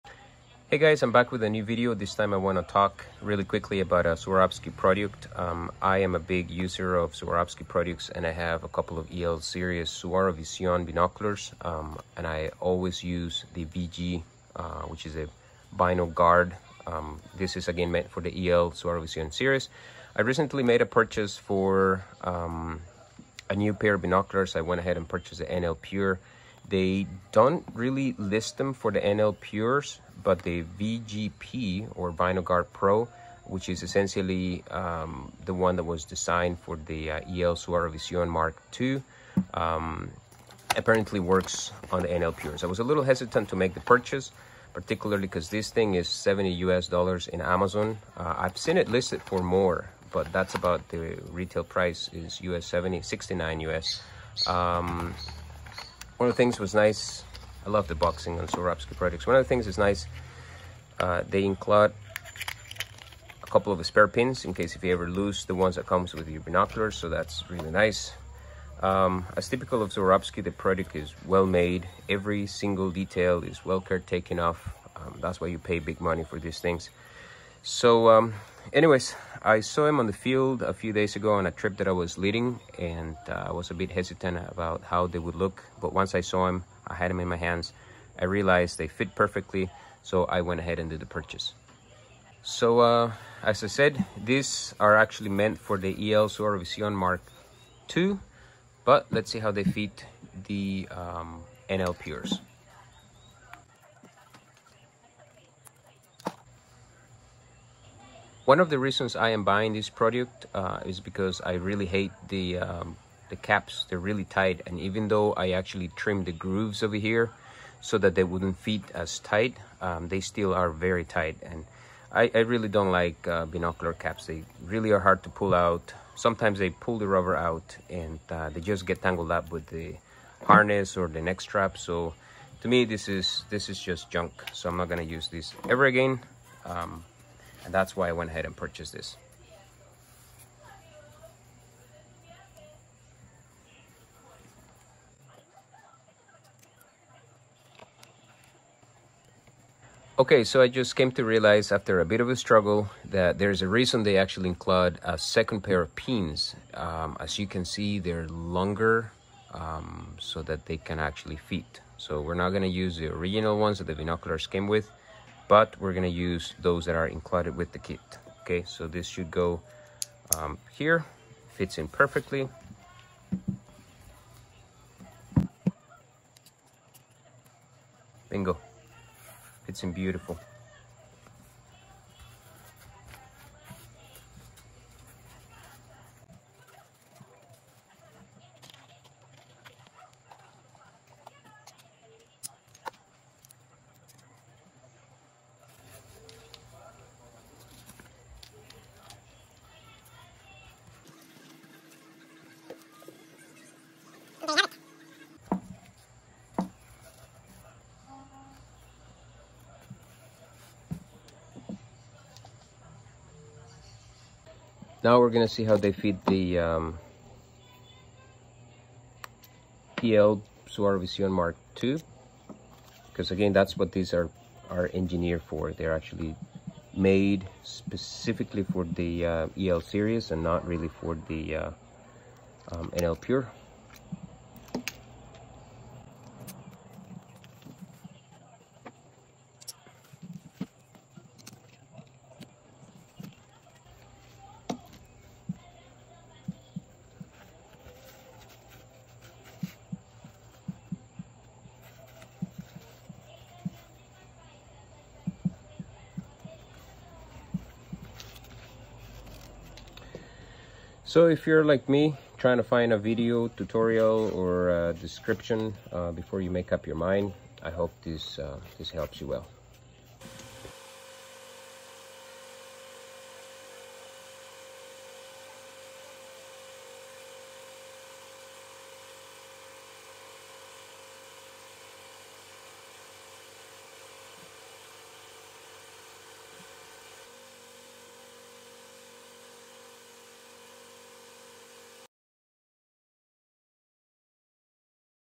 Swarovski Binocular cover — BG Pro Bino guard cover for NL pure
A short video on the Swarovski BGP binoguard pro cover for Swarovski NL Pure binoculars.